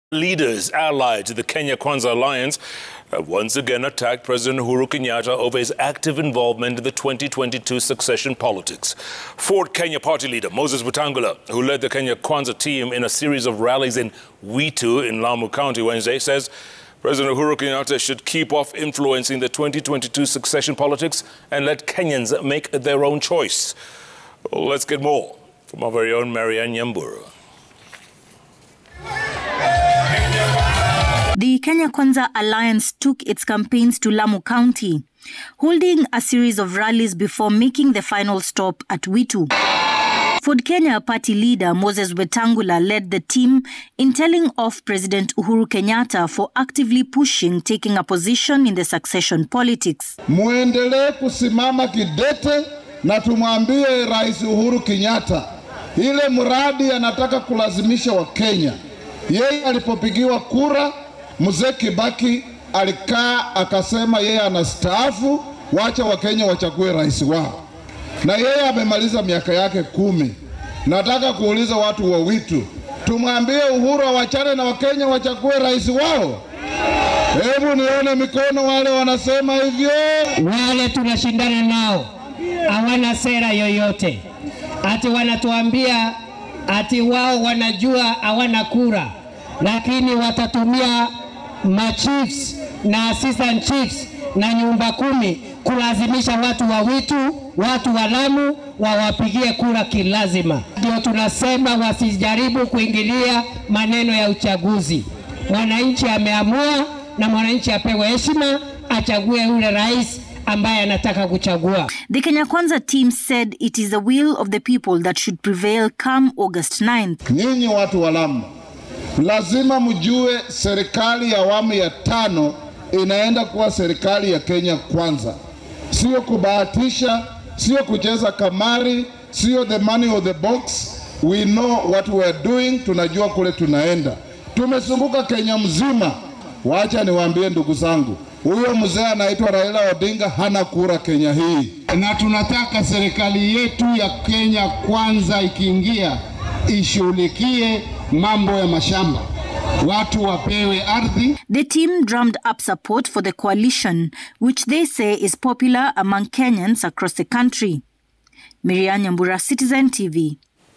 Moses Wetangula leads campaigns in Tana River and Lamu counties
Leaders hit out at handshake saying it has led to high cost of living
Leaders insist that Raila’s candidature is a government project